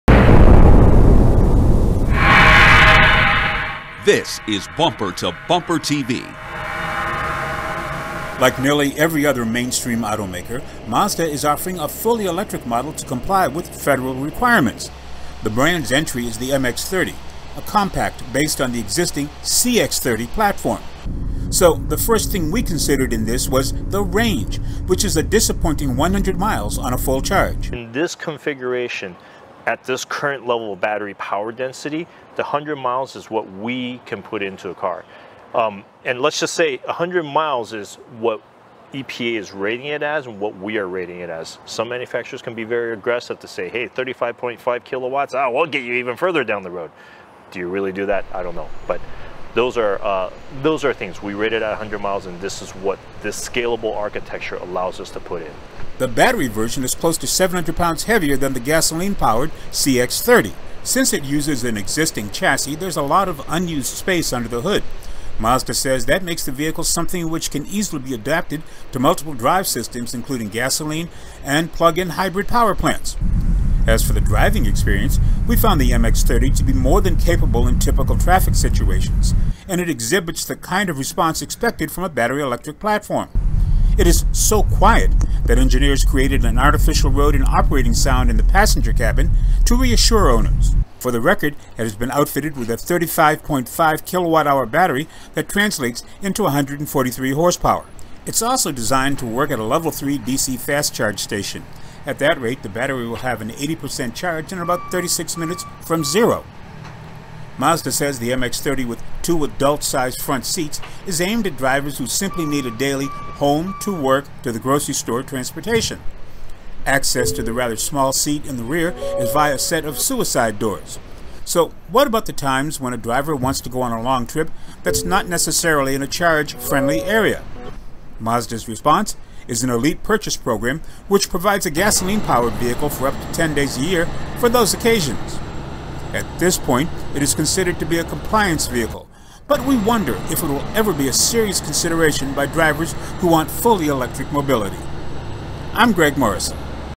Mazda MX30 Compact Stylish SUV - Walkaround Review by Greg Morrison
Watch a video introducing the Mazda MX30 compact crossover.